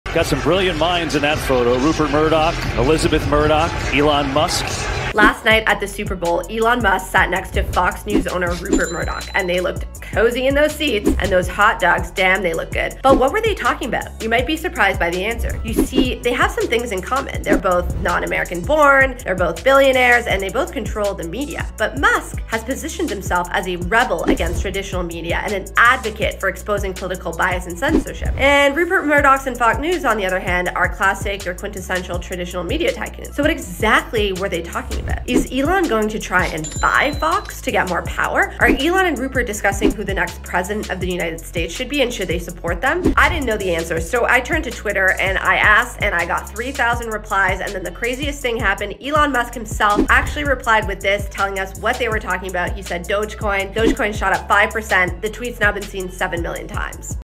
Elon Musk Seen Sitting with Rupert Murdoch and Elisabeth Murdoch at Super Bowl
Elon Musk had a prime seat at State Farm Stadium for Super Bowl LVII. Fox Sports cameras captured the maverick billionaire sitting with Fox Corp. and News Corp. chief Rupert Murdoch as well as his daughter, producer and investor Elisabeth Murdoch.